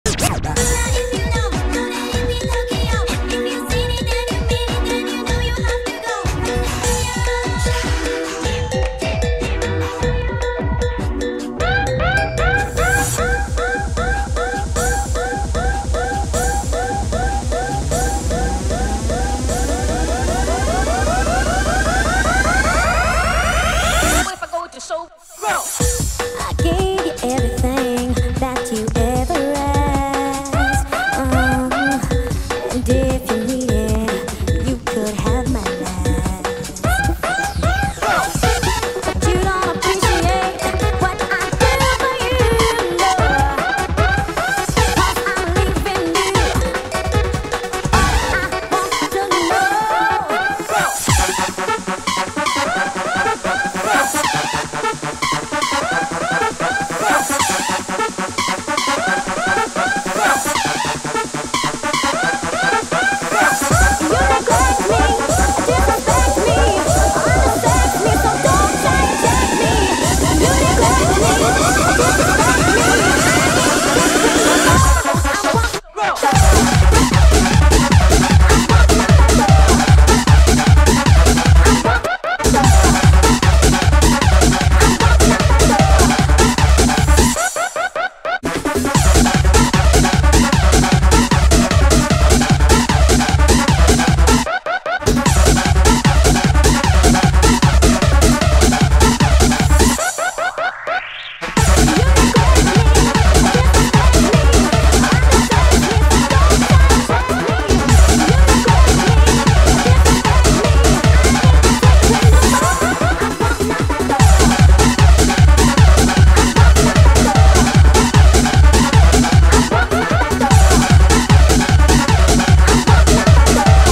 ROCKOLA X TOKIO | Cantadita SuperTop !!! Fuerza Remember
Hola Familia Remember hoy os traigo una Mezcl Tremenda con la  Cantadita de la Espectacular  Discoteca Rockola !!! Remember en estado Puro !!!